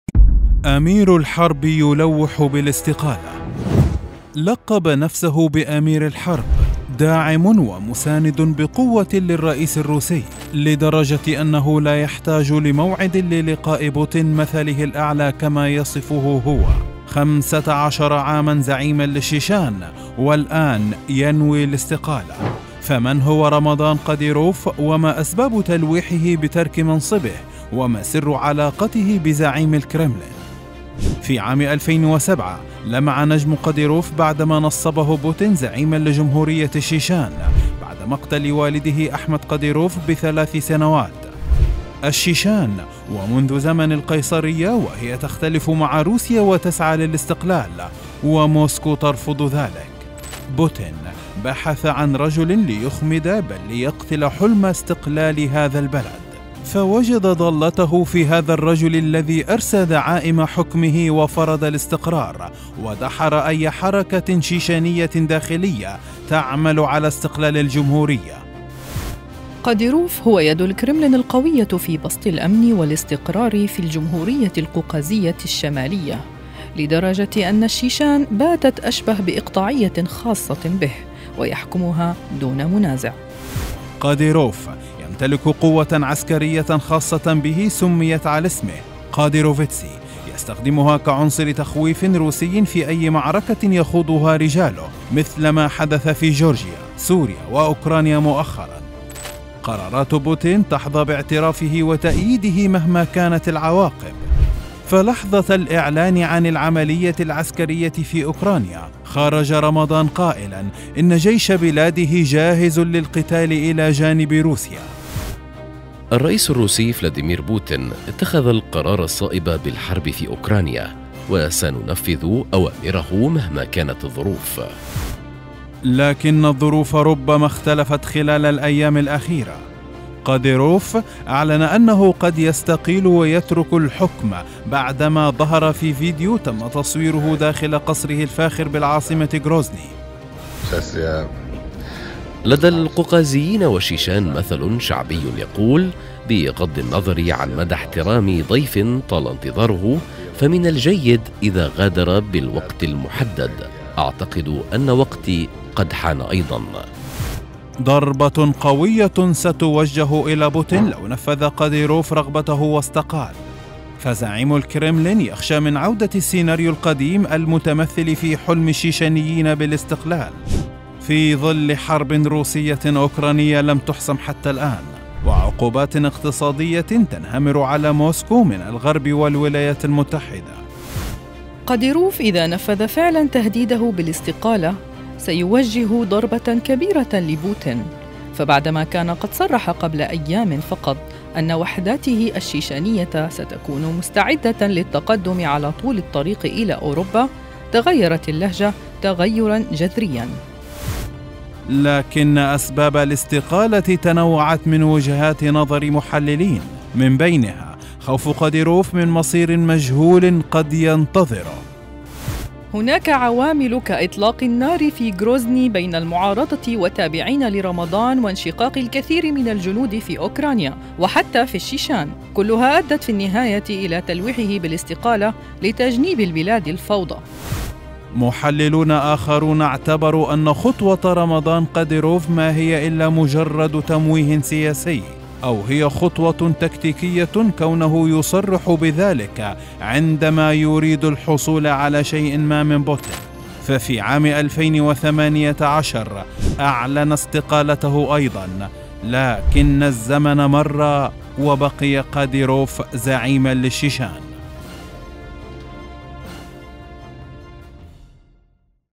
أسرار في الأخبار | رمضان قديروف "سيوجه ضربة لروسيا" في حال استقال من منصبه
أسرار_في_الأخبار | "ضربة قوية بوجه بوتين".. كشف أسرار تلويح رمضان_قديروف بتقديم استقالته من رئاسة جمهورية الشيشيان بعد 15 عاماً من الحكم